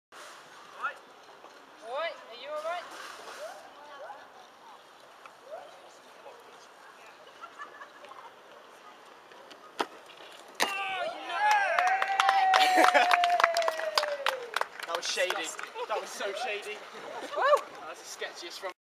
James Williams - Car flip.
James flips over his car :D